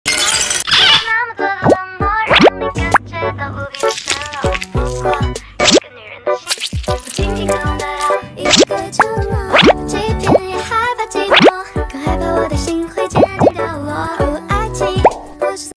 The drink cups - children can’t stop playing#shorts
The drink cups that can be DIYed freely are also so fun.